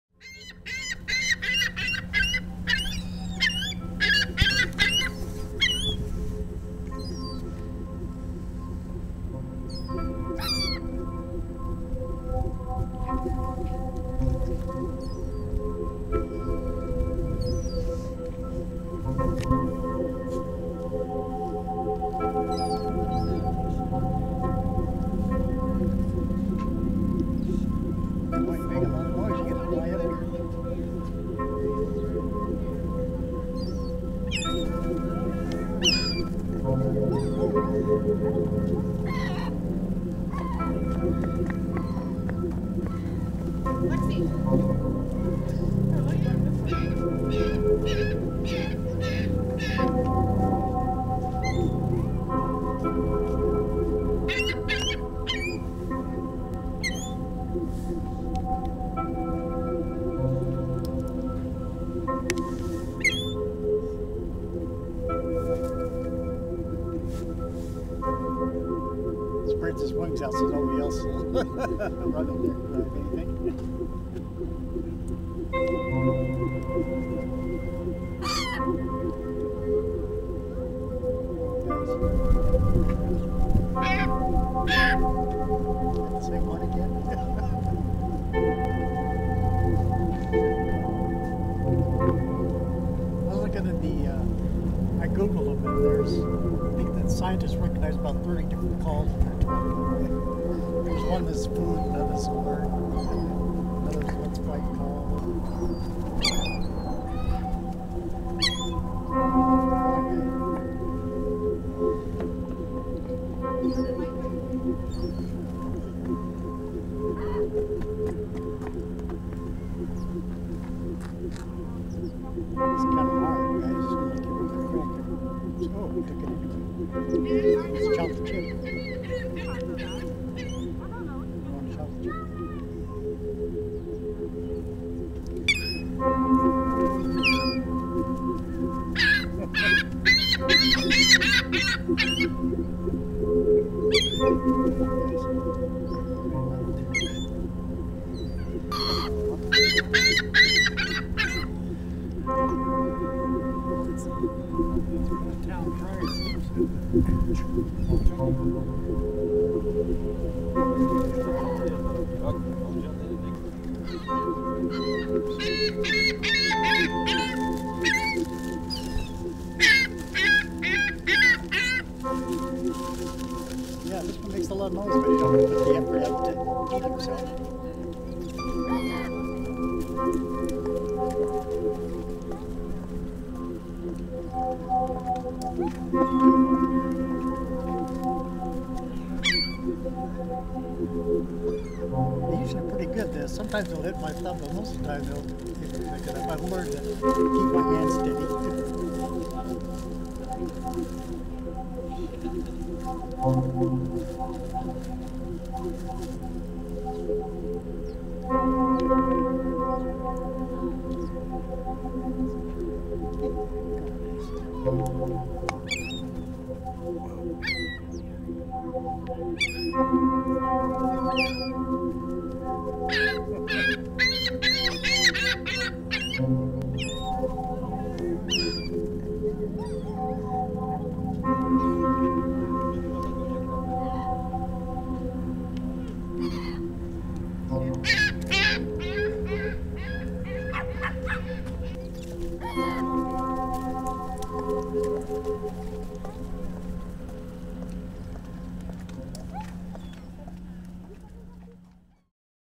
Battery Birds
I had just missed the 4:30 ferry and looped around to Battery Park. I met Jim, who comes to feed the birds a few times a week. It was beautiful to see the relationship he's built with the different birds.
The gulls are the most prominent and the pigeons try to get in there, but the starlings are the most delicate and sly in their approach to getting fed.
"They like crackers best", he told me, and it's easier to handle than bread. He liked taking pictures with an old canon film camera, with one hand, while holding the cracker steady with his other hand.

soundscape created with
Ciruli, the generative sound instrument